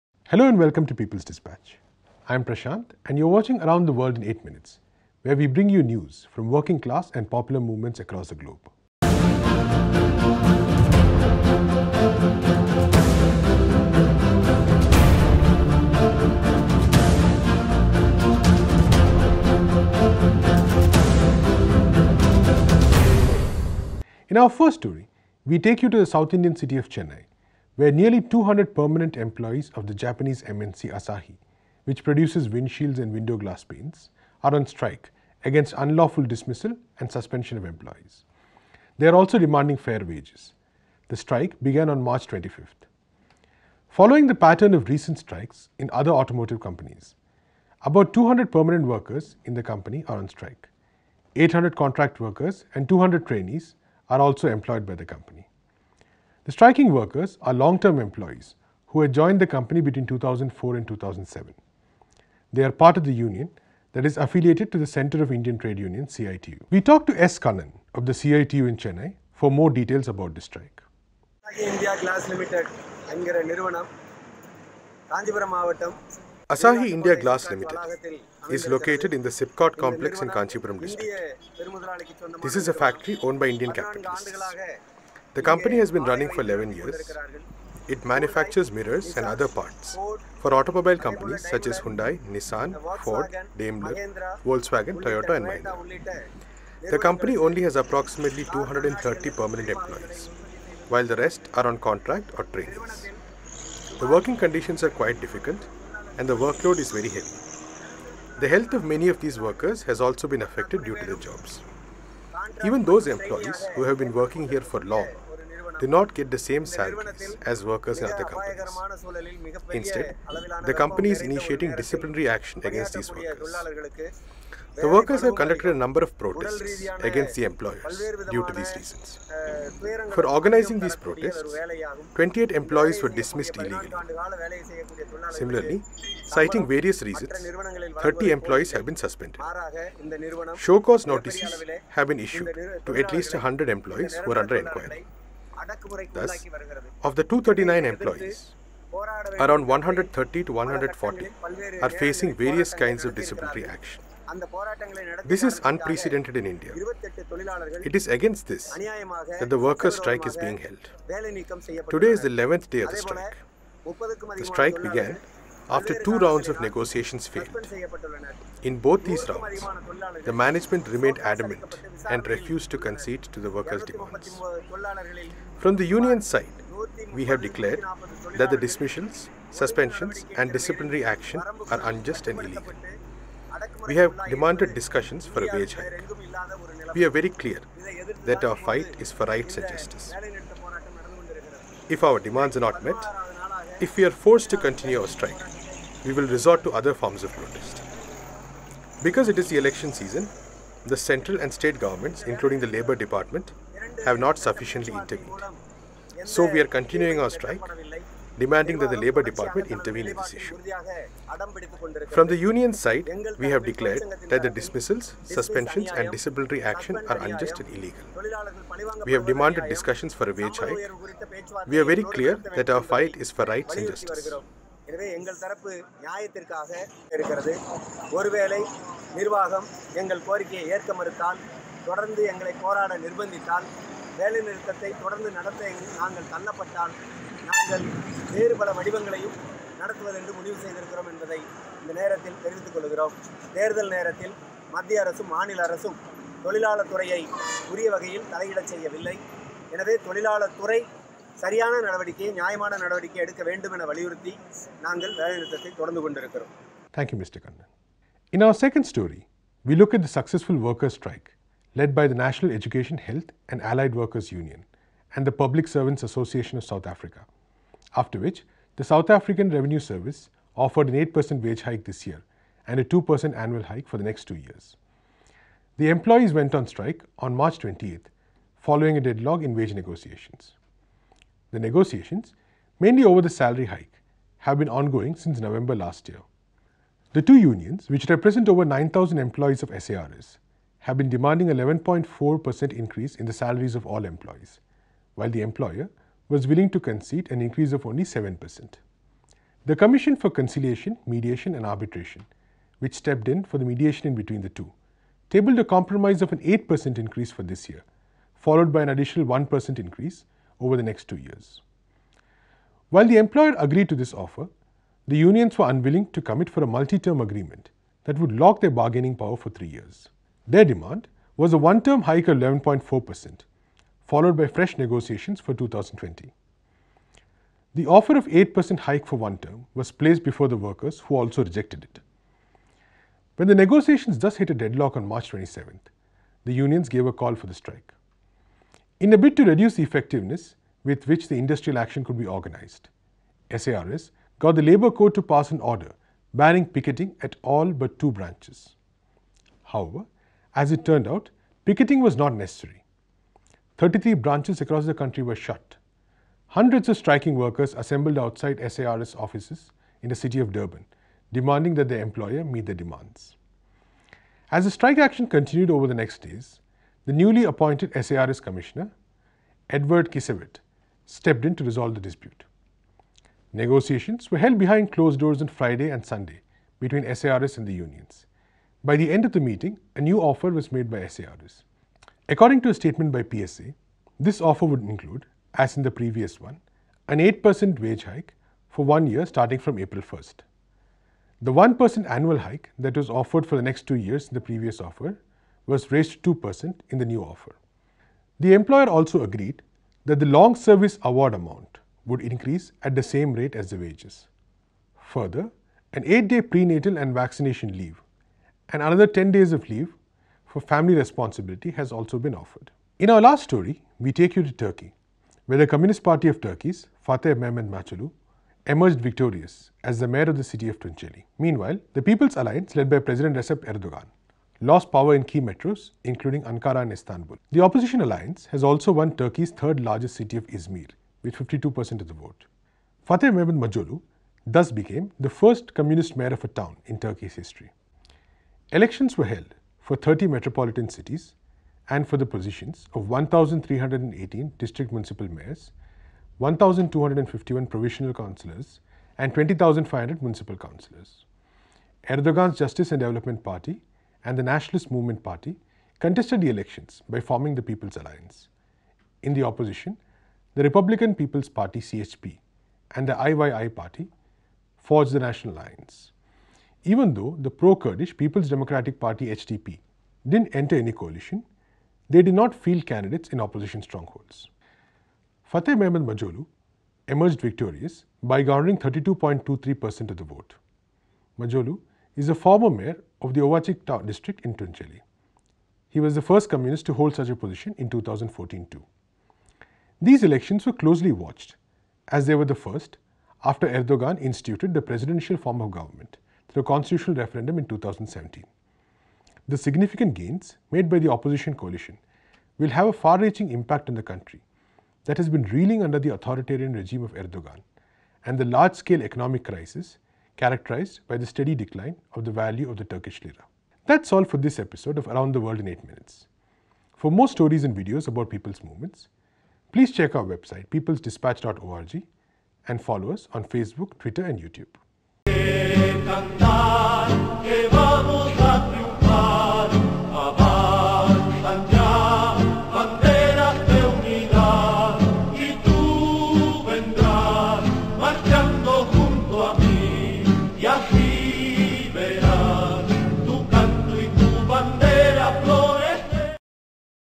Around the World in 8 Minutes: Episode 15 | International News on Working Class & Popular Struggles
In this episode, Peoples Dispatch brings you reports from the workers’ strike in the southern Indian state of Tamil Nadu, and South African allied and public service workers’ industrial action. We also look into the victory of the first communist mayor in Turkey’s local body polls.

(Around the world in 8 minutes is a show from Peoples Dispatch where we bring you news from people’s movements and organizations across the globe.)